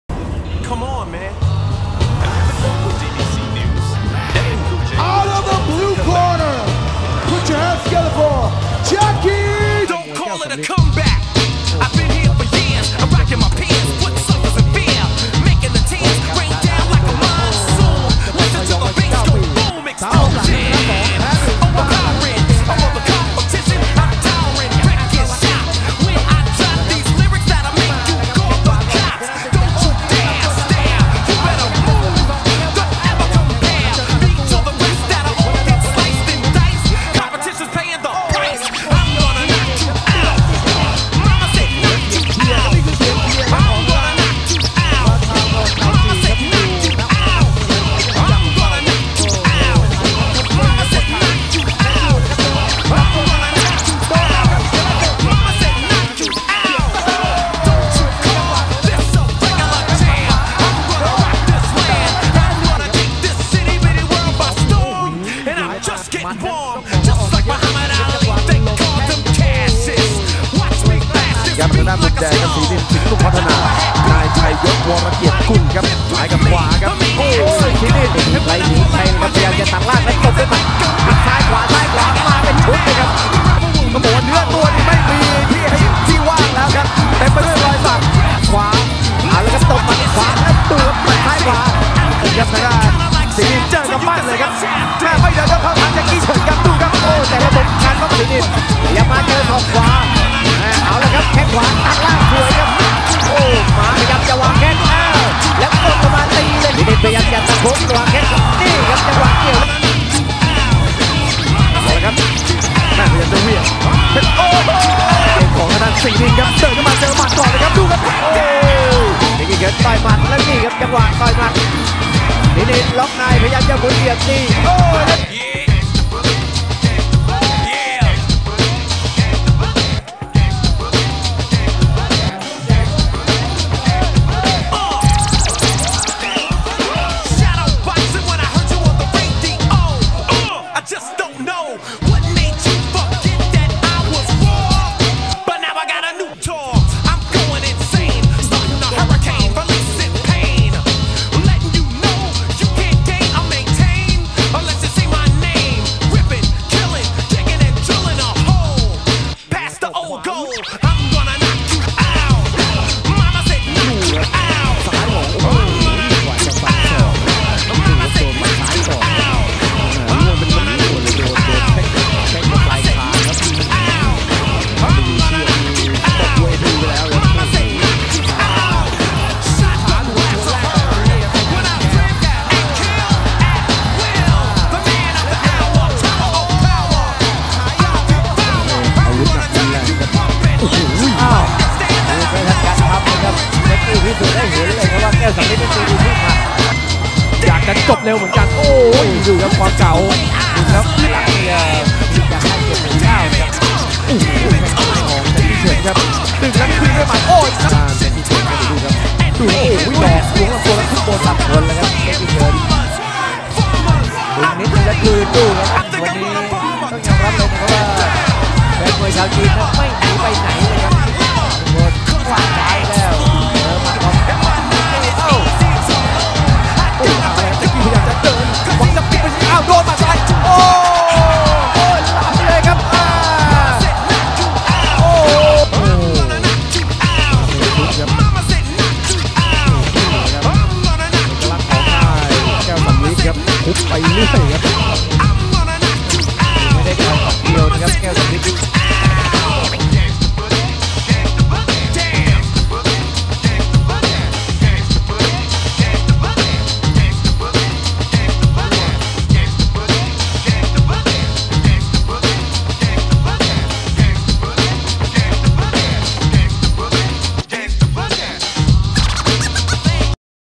Jacky Chan highlights - Muay Thai
my recent fights in Thailand hope you enjoy
Max Muay Thai